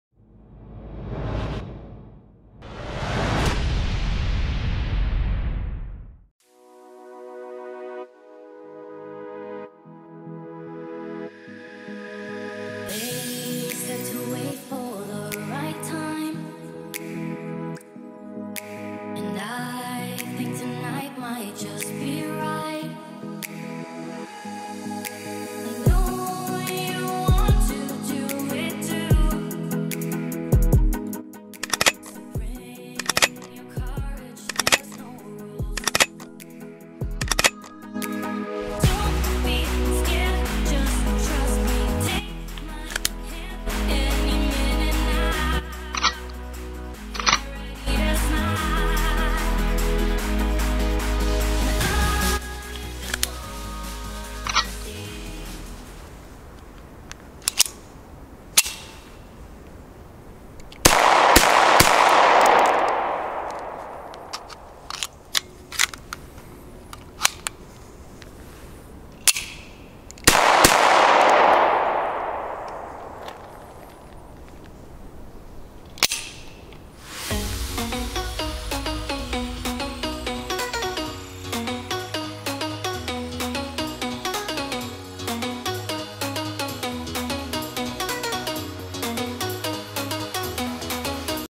Zoraki 914 Schreckschuss | Chrom-Holz | Schusstest + Magazinwechsel | Pyro Knarre [FullHD]
Was hat das mit Feuerwerk zu tun? Unzwar kann man mit einer Schreckschusswaffe auch Pyromunition mit Hilfe eines kleines Aufsatzes (Pyrobecher) verschießen. Hoffe Euch gefällt mein Video zur Zoraki 914.
* Kamera, Camcorder, Videoequipment:

* 2x BESTER Brenner (Sturmfeuerzeug)

Pyro-Equipment:

2 Kanal-Funk-Zündanlage
4 Kanal-Funk-Zündanlage
10 Kanal-Funk-Zündanlage

KEEP WATCHING.